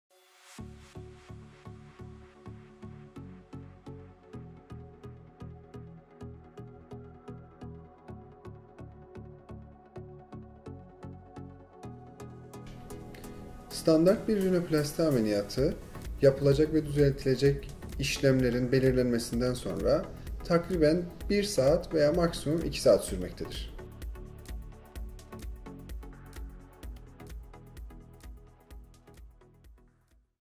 Rinoplasti ameliyatı kaç saat sürer? - Op. Dr. Hüseyin Kandulu
Op. Dr. Hüseyin Kandulu, "Rinoplasti ameliyatı kaç saat sürer?" sorusunun yanıtını veriyor.